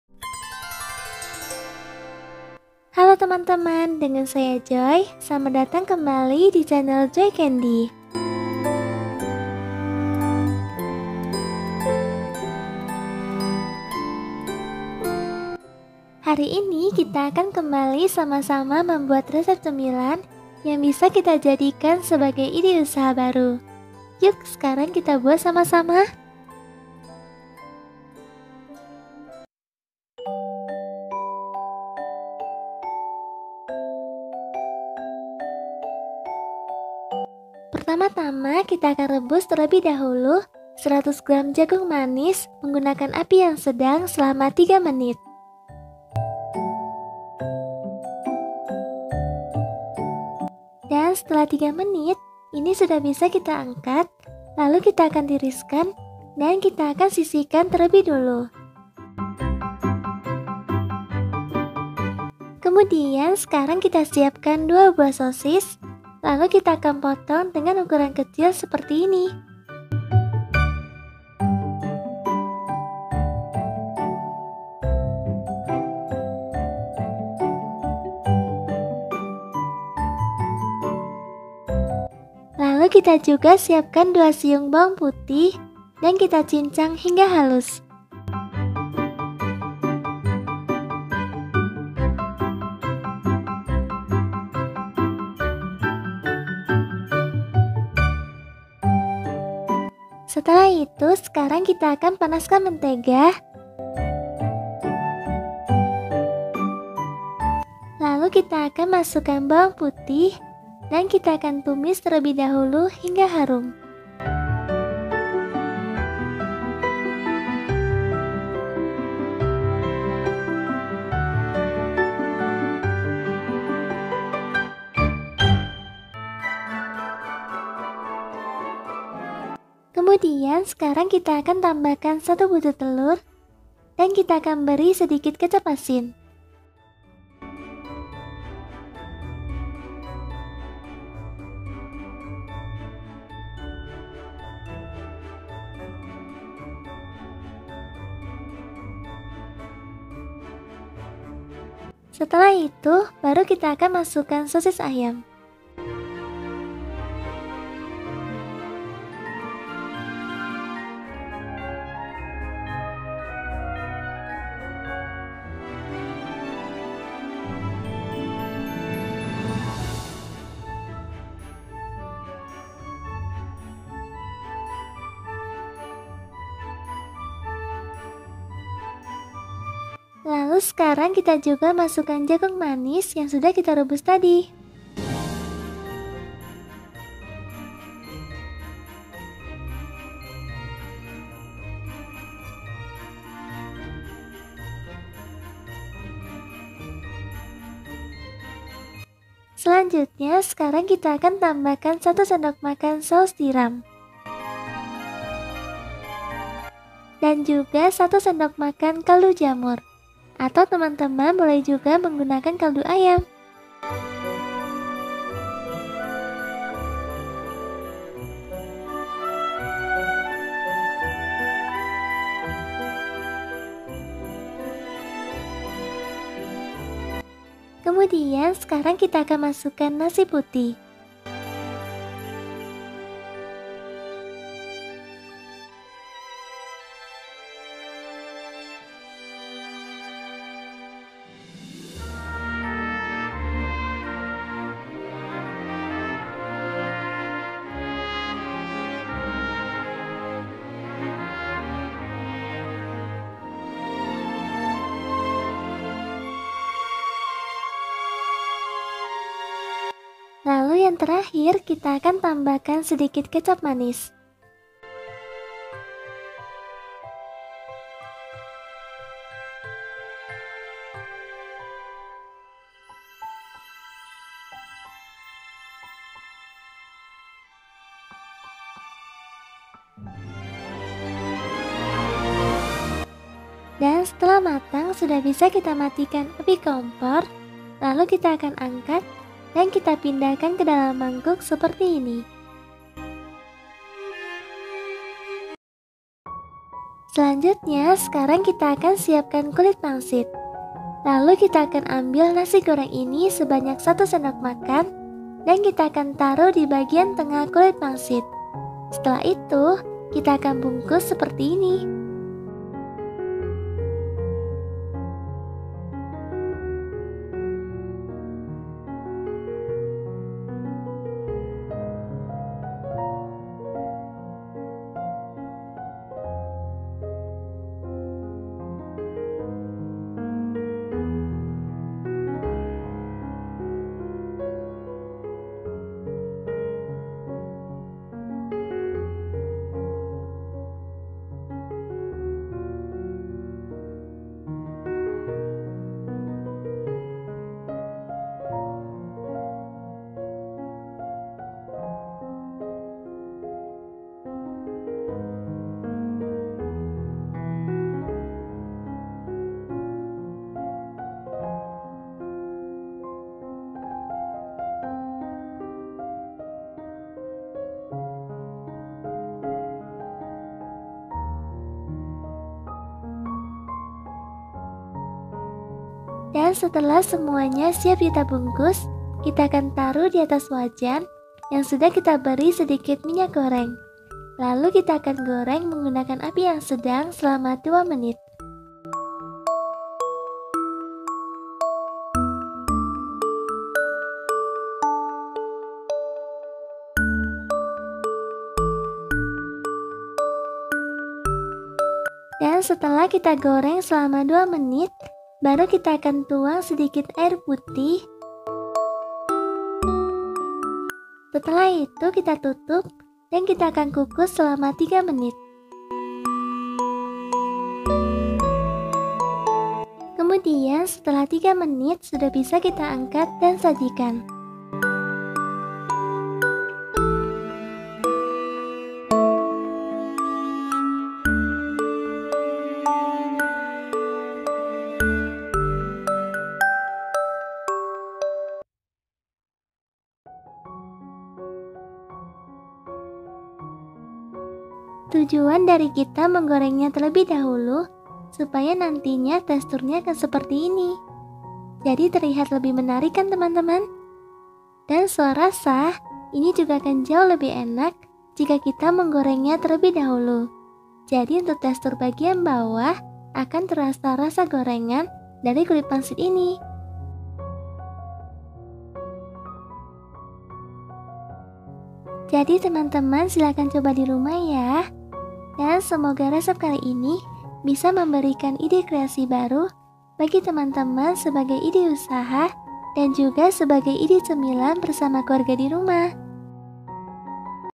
DIJAMIN BELUM ADA YANG JUAL | 点心レシピ | FRIED RICE DUMPLINGS RECIPE
Halo semuanya, kali ini kita akan membuat ide bisnis terbaru dan dijamin belum ada yang jual, cara buatnya sangat mudah dan rasanya sangat enak.
Yuk kita buat sama2...🤗

Bahan :
100 gr Jagung Manis (2 buah jagung) Rp 2000
2 buah Sosis Ayam Rp 2000
2 siung Bawang Putih Rp 500
1 sdt Mentega Rp 500
1 butir Telur Rp 1500
1 sdm Kecap Asin (Sesuai Selera) Rp 500
1 sdm Saus Tiram Rp 500
1 sdm Kaldu Bubuk (boleh rasa apa saja) Rp 500
1 sdm Kecap Manis (Sesuai Selera) Rp 500
250 gr Nasi Putih Rp 2000
Kulit Pangsit Rp 5000

Modal bahan +/- 18.000 (sudah termasuk minyak dan gas)
dan utk takaran bahan ini aku dapatkan 33 buah.
jadi modal perbuah +/- Rp 550.
(ini harga bahan didaerahku yah teman2)

Terima kasih buat teman2 yang sudah mau menonton resep kali ini, semoga bisa bermanfaat yah.

♡SELAMAT MENCOBA♡